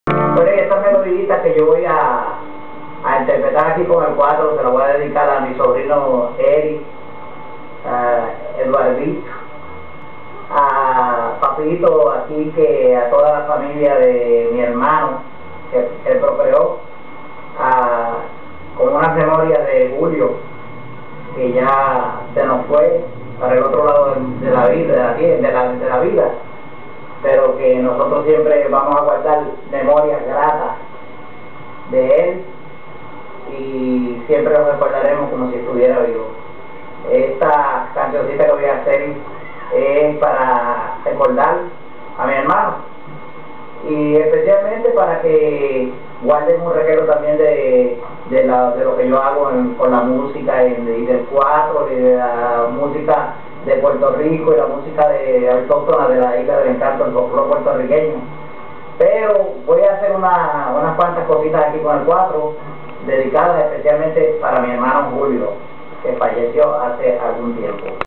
Yomo Toro Part 1 DSCN3749.MOV
A short intro clip my uncle did in 1996 just after my dad, his brother Julio passed away. Quality isn't great as I recorded off my tv but will serve for now. the actual video is digital and runs about a half hour with Yomo breaking in between songs to talk about his work from time to time and discuss different types of music.  Since he has passed away, no other Toro or family member plays music professionally. He was the only one, and the best.